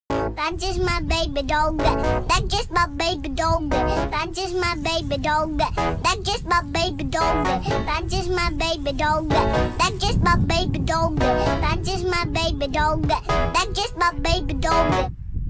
Cutest cocker spaniel puppy enjoying an autumn walk
Cute cocker spaniel puppy enjoying an autumn walk